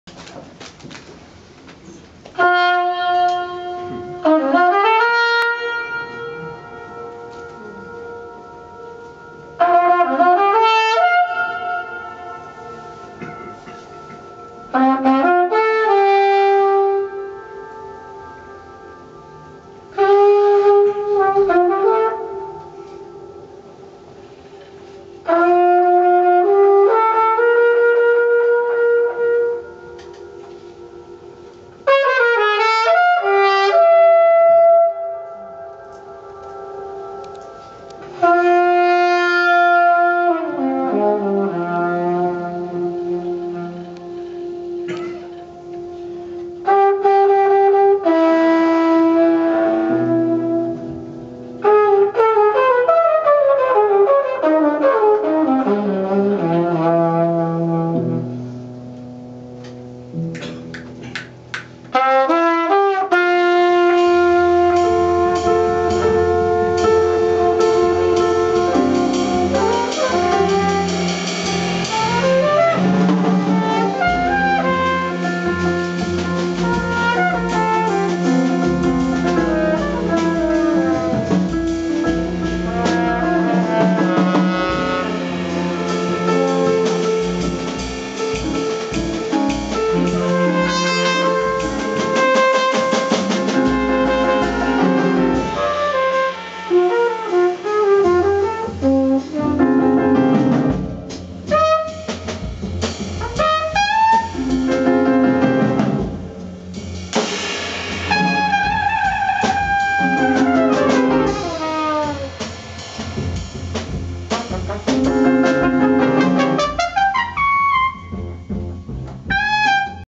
Tobias Meinhart Quartet & Ingrid Jensen im Bamberger Jazzclub
Live im Bamberger Jazzclub am 23. September 2017. Im Video das letzte Stück des Abends.
Mitwirkende: Tobias Meinhart (ts), Ingrid Jensen (tp), Yago Vazquez (p), Phil Donkin (b), Jesse Simpson (dr)
Tolles Konzert mit einer phänomenalen Ingrid Jensen.